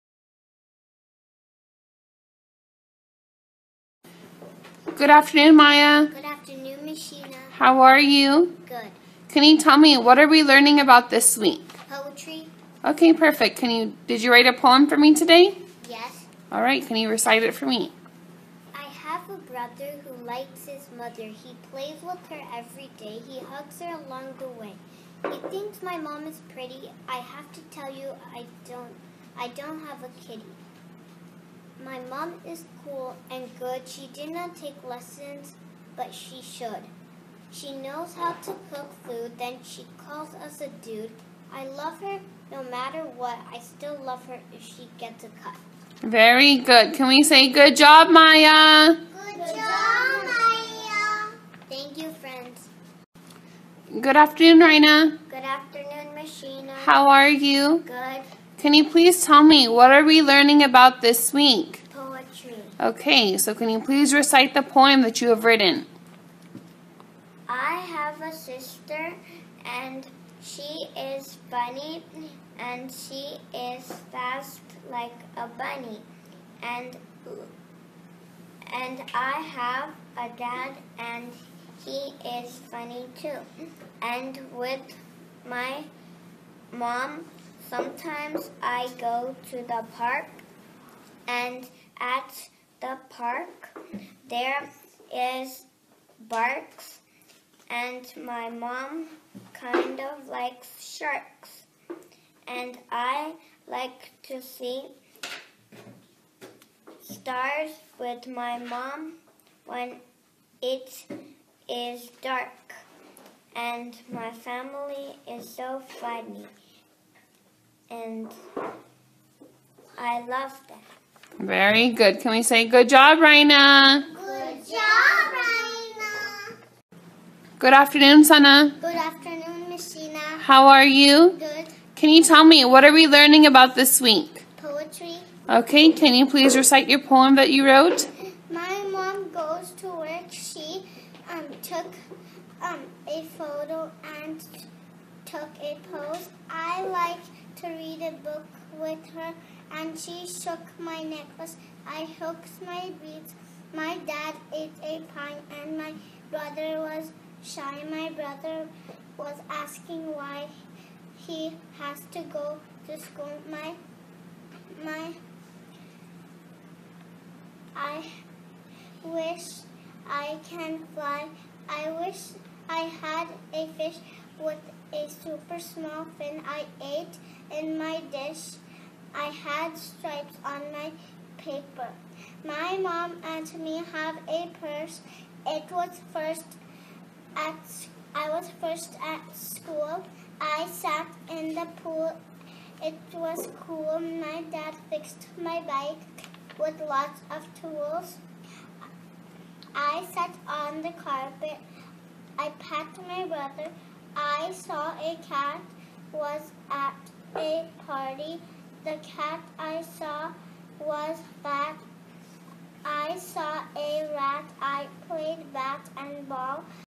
Poetry Week at Genius Kids Pleasanton
Fun week learning and creating our own poems